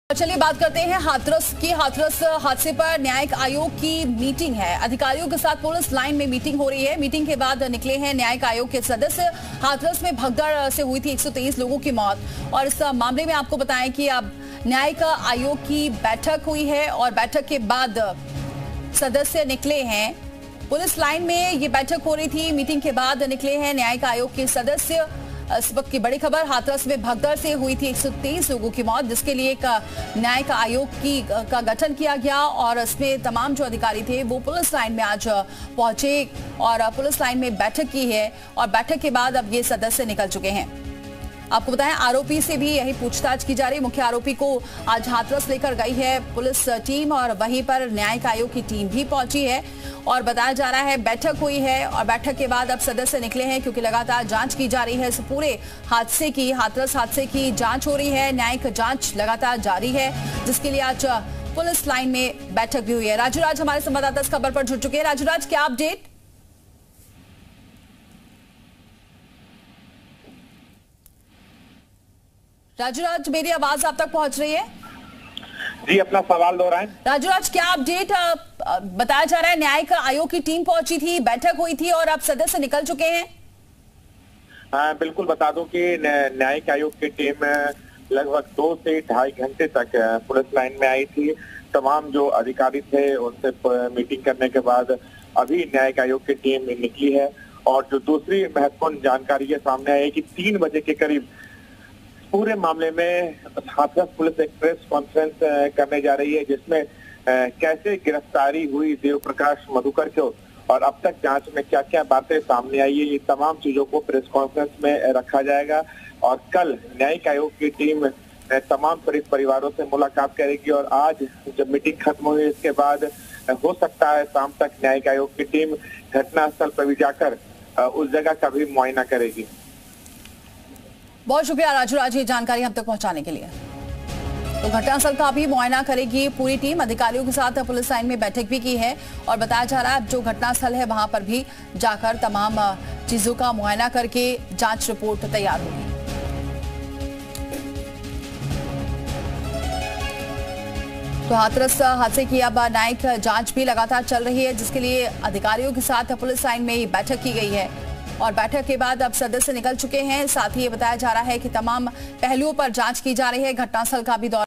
Hathras Satsang Breaking News: हाथरस हादसे पर न्यायिक आयोक की मीटिंग | Judicial Commission Meeting
Hathras Accident: हाथरस हादसे पर न्यायिक आयोक की मीटिंग, अधिकारियों के साथ पुलिस लाइन में मीटिंग, मीटिंग के बाद निकले न्यायिक आयोग के सदस्य, हाथरस में भगदड़ से हुई थी 123 लोगों की मौत